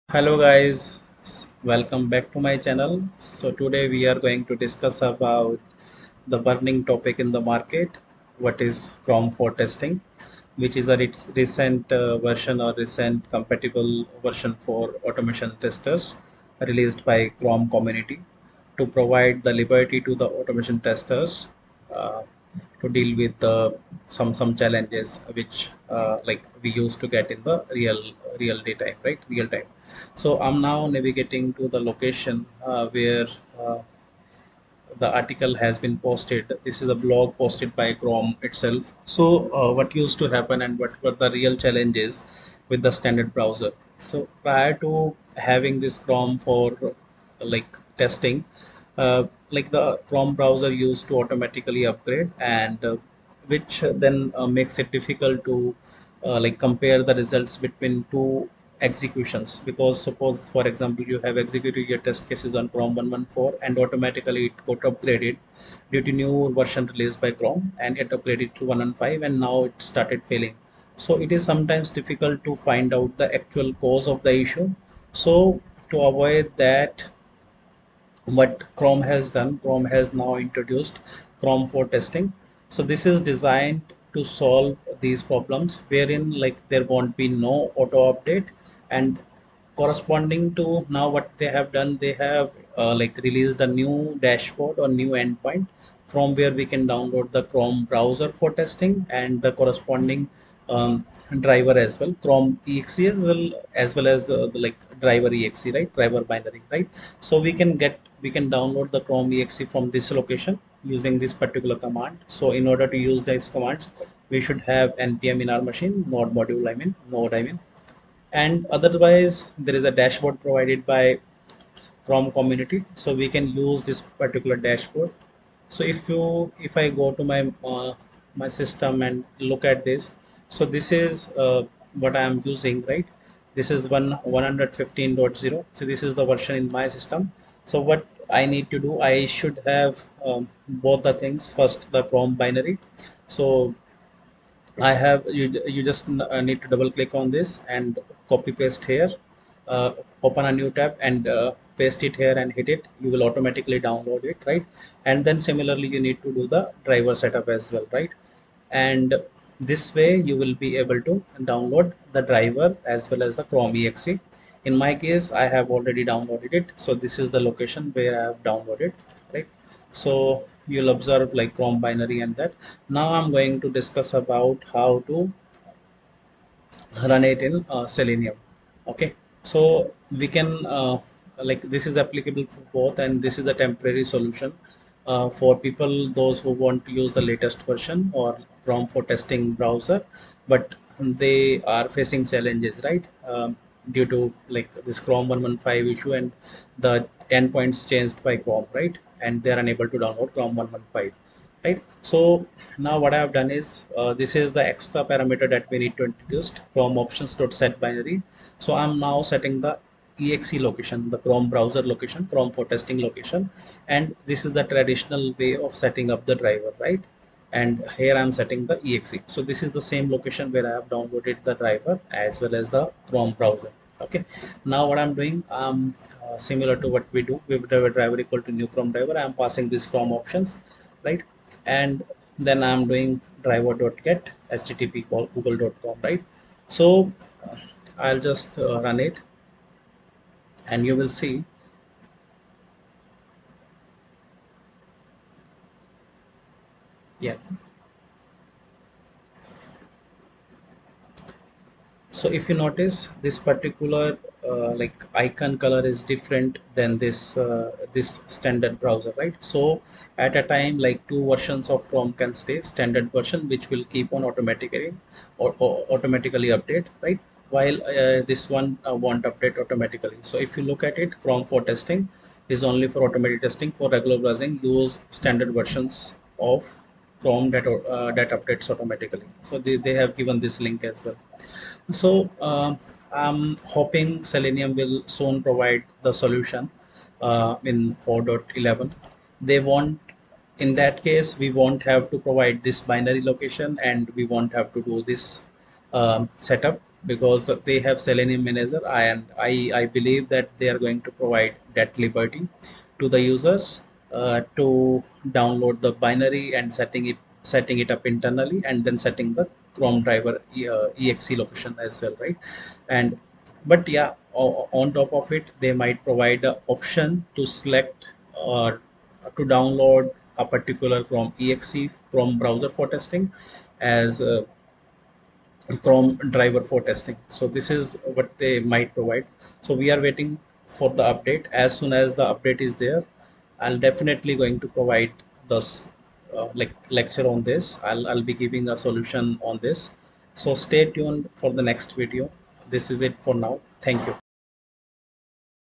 2. What is Chrome For Testing, How to Handle chrome 115 using Chrome for testing (Cft)
Hello Everyone,

In this video we discussed about following topics:
1. What is Chrome for Testing (Cft) : Chrome 115 onwards
2. What were the key challenges prior to chrome for testing
3. How to download chrome for Testing browser and driver exe
4. How to run test cases in Chrome for testing Browser

Useful links:
1. What is chrome for Testing
2. Link to download Chrome for Testing Browser and Driver binaries:

In the next lecture we are going to discuss about Selenium 4.11 and how Selenium Manager manages downloading of Browser and Binaries.